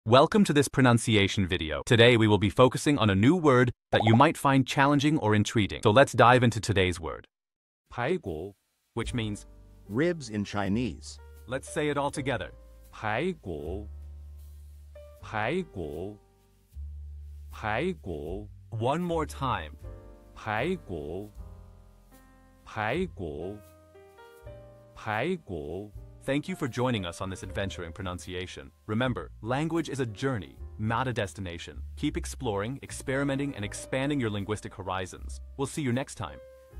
How to pronounce pái gǔ | 排骨 (Ribs in Chinese)
Master the Pronunciation of 'pái gǔ | 排骨' - which means : Ribs in Chinese 📝 with @PerfectPronunciationChinese 🎓🗣️ - [Your Guide to Chinese]

Learn to pronounce 'pái gǔ' perfectly in Chinese with our easy and engaging tutorial by @PerfectPronunciationChinese! 📖📽️ In this comprehensive video, we provide you with native speaker examples 🗣️👂 and phonetic breakdowns 🎵 to ensure you get the pronunciation just right.